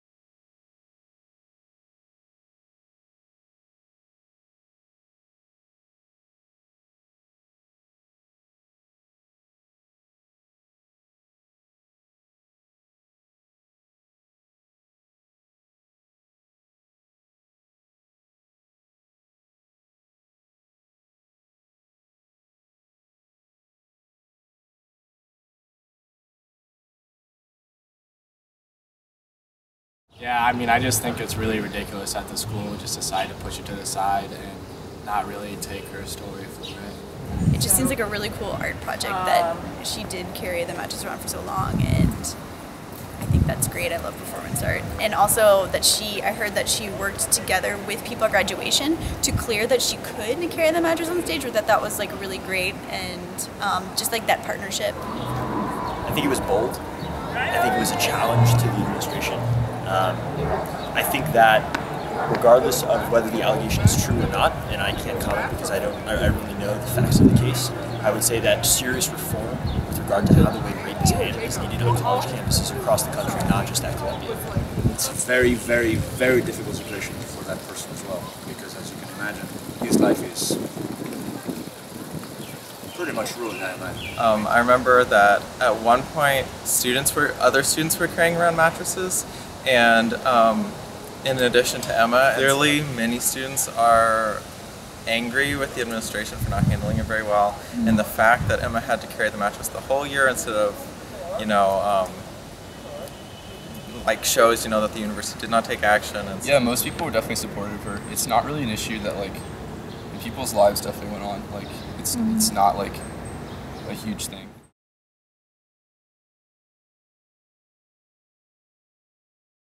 Students Sound Off On Emma Sulkowicz Mattress Performance Piece
Columbia University students discuss the university's response to the rape allegations of Emma Sulkowicz, who brought a mattress to her May 2015 graduation as a protest performance piece.